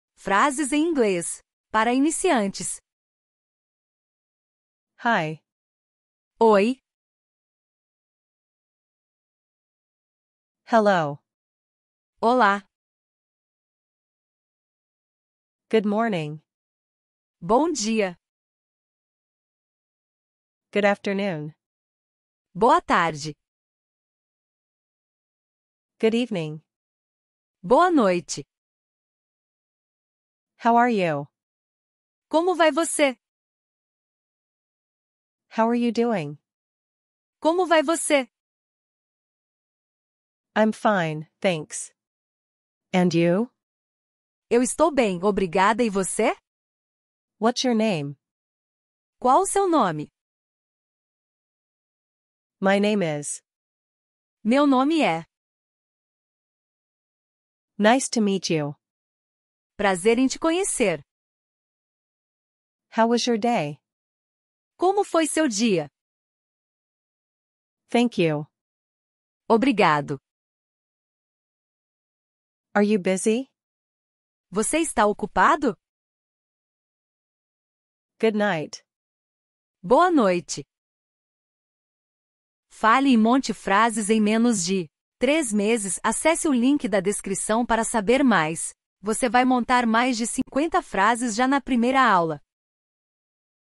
Frases em Inglês para Iniciantes | Frases curtas em Inglês para Iniciantes
Aprenda como falar as frases em Inglês para Iniciantes, você vai aprender frases simples em inglês para iniciantes com pronúncia e também frases curtas em Inglês para Iniciantes com esta aula.

Aprenda também como falar Frases em Inglês para Iniciantes e montar frases de forma rápida e prática com um professor fluente, acesse o link para saber mais.